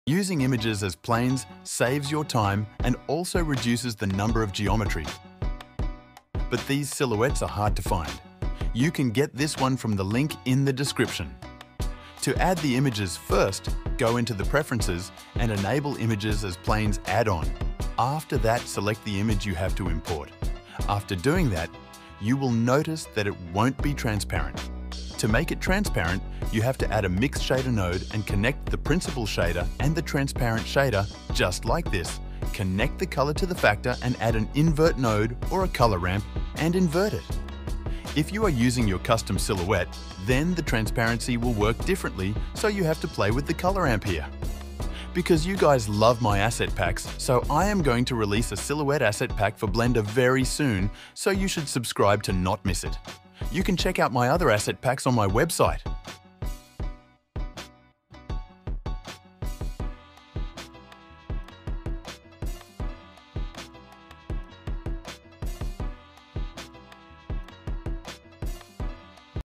How To Use Images As Characters In blender
00:00 INTRO
00:10 HOW TO IMPORT
00:27 HOW TO SETUP MATERIAL
00:43 FREE ASSET PACK